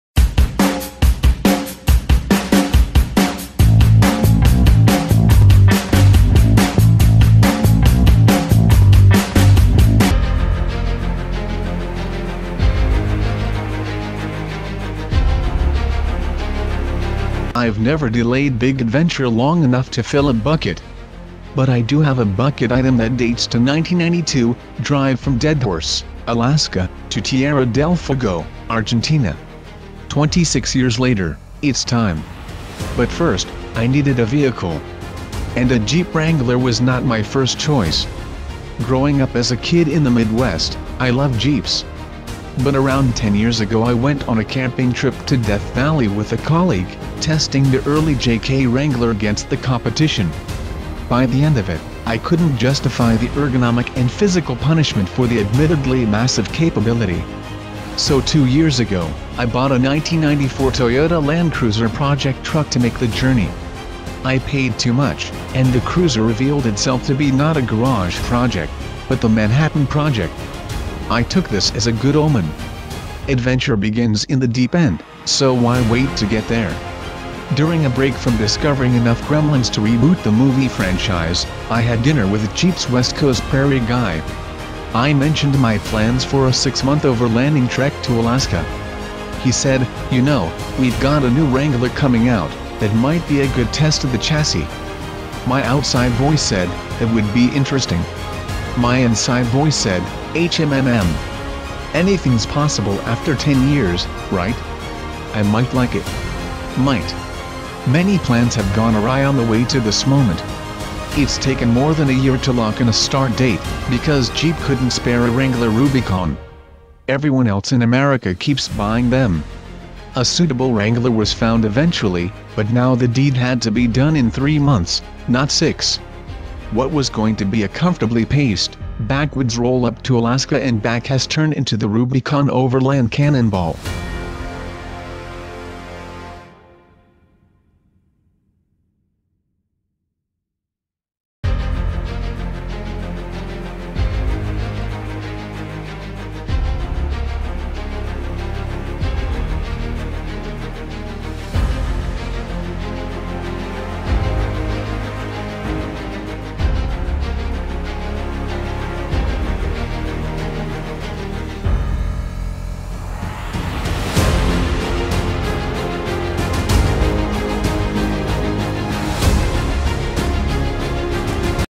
2018 Jeep Wrangler Rubicon Alaska Cannonball
2018 Jeep Wrangler Rubicon Alaska Cannonball
I've never delayed big adventure long enough to fill a bucket. But I do have a bucket item that dates to 1992: drive from Deadhorse, Alaska, to Tierra del Fuego, Argentina. Twenty-six years later, it's time. But first, I needed a vehicle. And a Jeep Wrangler was not my first choice.

Growing up as a kid in the Midwest, I loved Jeeps. But around 10 years ago I went on a camping trip to Death Valley with a colleague, testing the early JK Wrangler against the competition. By the end of it, I couldn't justify the ergonomic and physical punishment for the admittedly massive capability. So two years ago, I bought a 1994 Toyota Land Cruiser project truck to make the journey. I paid too much, and the Cruiser revealed itself to be not a garage project, but the Manhattan Project. I took this as a good omen. Adventure begins in the deep end, so why wait to get there?

During a break from discovering enough gremlins to reboot the movie franchise, I had dinner with Jeep's West Coast PR guy. I mentioned my plans for a six-month overlanding trek to Alaska. He said, "You know, we've got a new Wrangler coming out — that might be a good test of the chassis." My outside voice said, "That would be interesting." My inside voice said, "Hmmm." Anything's possible after 10 years, right? I might like it. Might.

2018 Jeep Wrangler Rubicon Alaska Cannonball